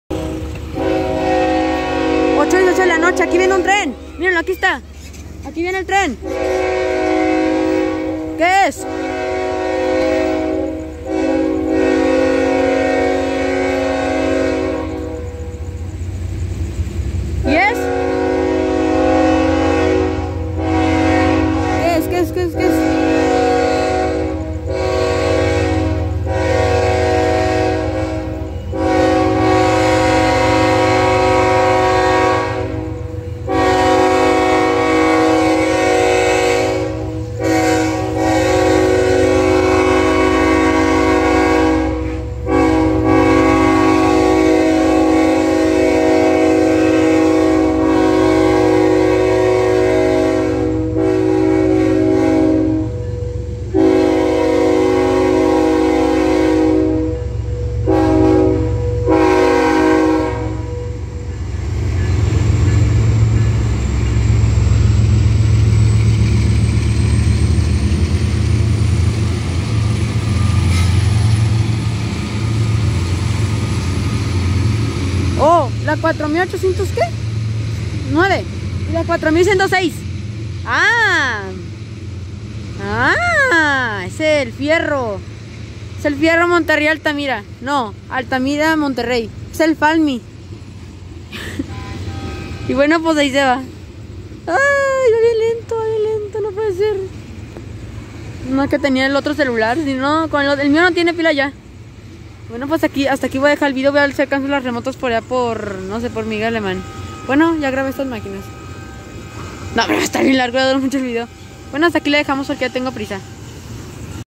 Tren ferromex 4809 4106
Este tren lo vi en San Nicolás con el Fierro Altamira-Monterrey FALMY con 2 máquinas y con gondolas y unas balasteras con Rumbo hacia los patios KCSM para ser llevados hacia los patios ferromex proviene de Altamira Monterrey la 4106 es la favorita de FXE 4106 y no me quedé tanto debido a que tenía prisa y además el tráfico intenso de carros que había era una ES44AC y una SD70ACe Grabado Viernes 24 Septiembre 2021 a las 8:17 PM Saludos a todos y al maquinista y su tripulación